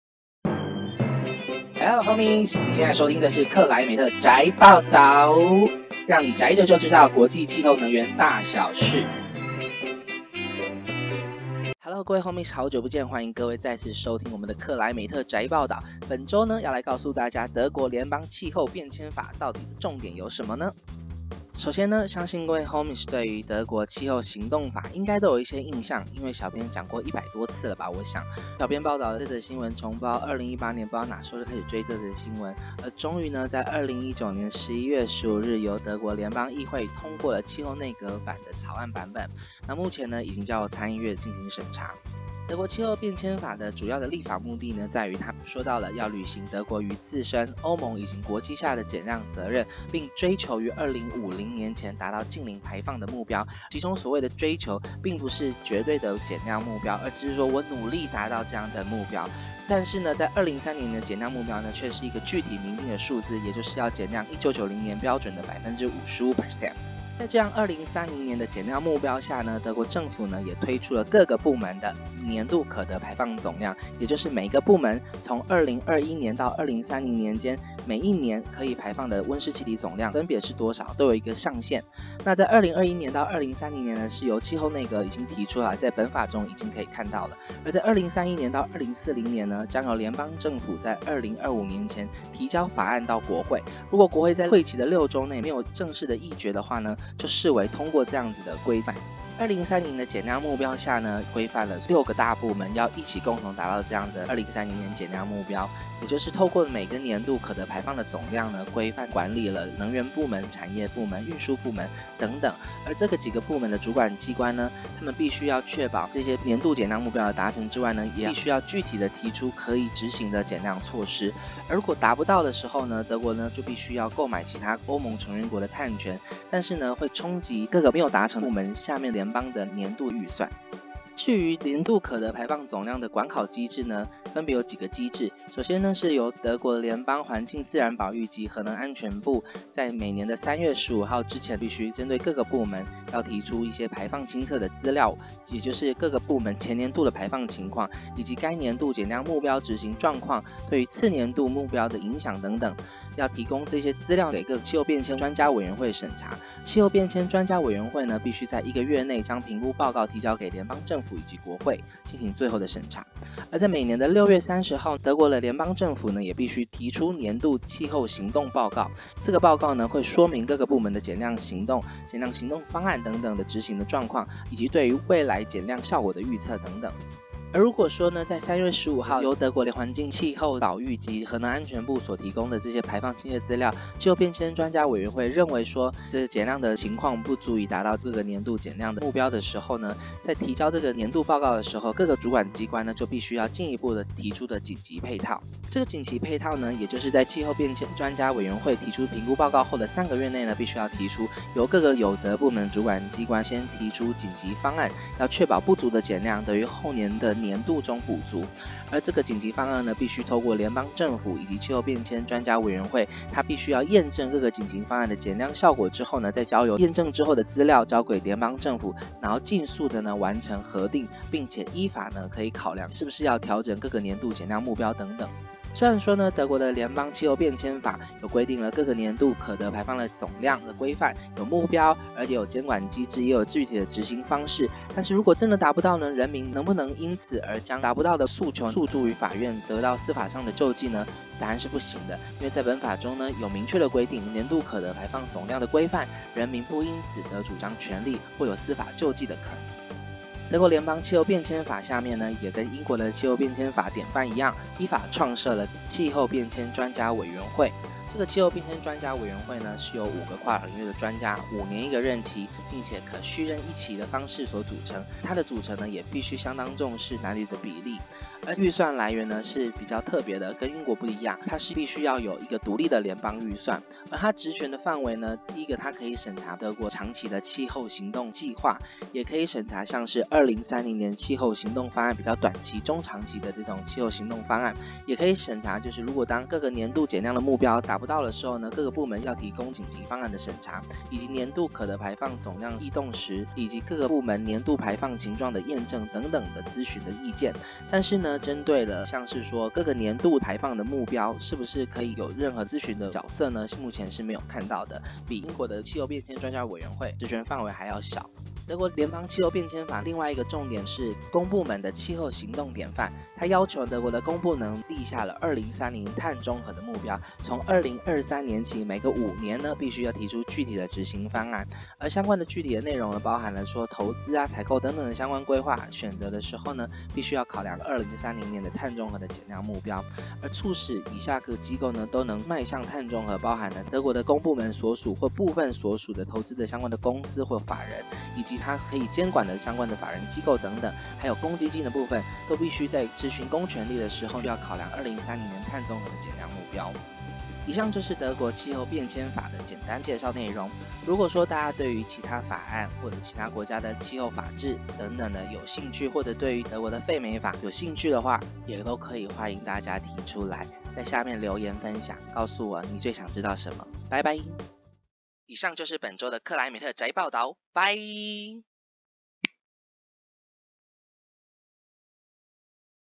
【台大風險中心】克萊美特宅報導_EP12
Hello Hommies~ 克萊美特宅新聞，讓你宅著就知道國際氣候能源大小事 EP12
主要新聞內容擷取時間：20191115~20191118
 ◎新聞來源
CLEW.

更多閱讀：風險中心臉書網誌【德國2030年氣候行動方案】